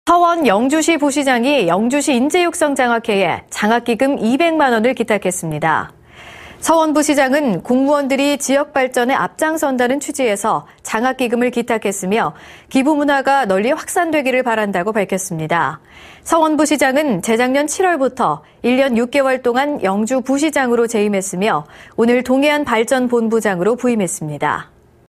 [안동MBC뉴스] 서원 영주부시장, 장학기금 기탁
2016/01/05 17:09:33    작성자 : 정윤호

서원 영주시 부시장이 영주시인재육성장학회에
장학기금 2백만원을 기탁했습니다.
◀ＥＮＤ▶

서원부시장은 공무원들이 지역발전에
앞장선다는 취지에서 장학기금을 기탁했으며,
기부문화가 널리 확산되기를 바란다고
밝혔습니다.

서원 부시장은, 재작년 7월부터
1년 6개월동안 영주부시장으로 재임했으며,
오늘(5일) 동해안발전본부장으로 부임합니다.